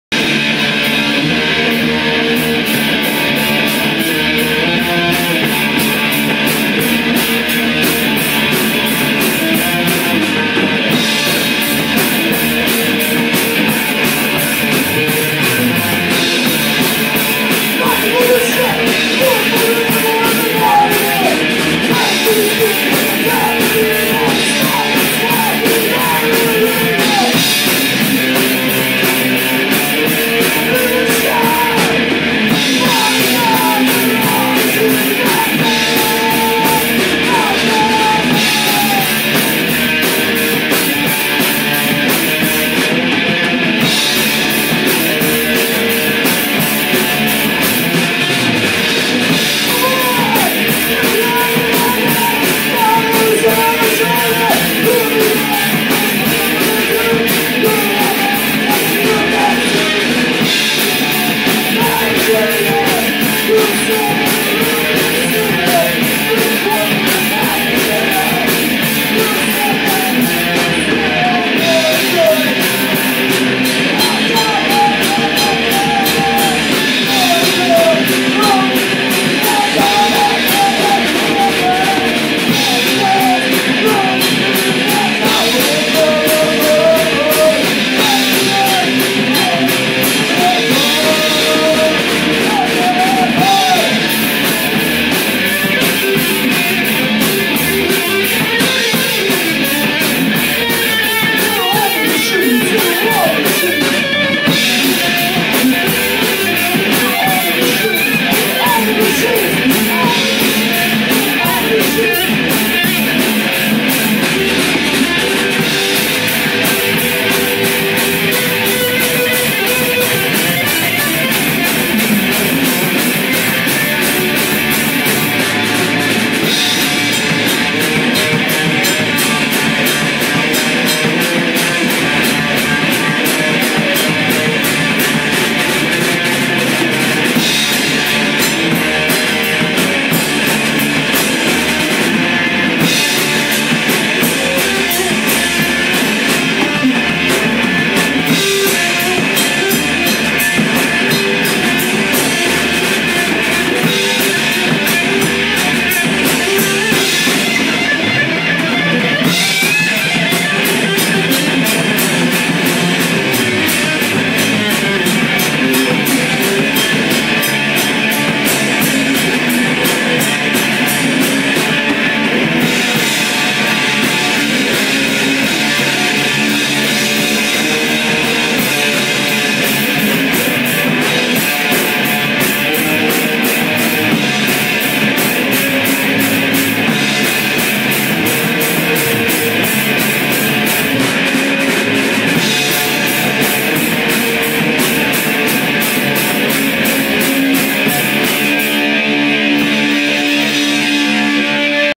The Eyesores
Performing at Johnny V's In Yo Face Punk Fest November 15, 2014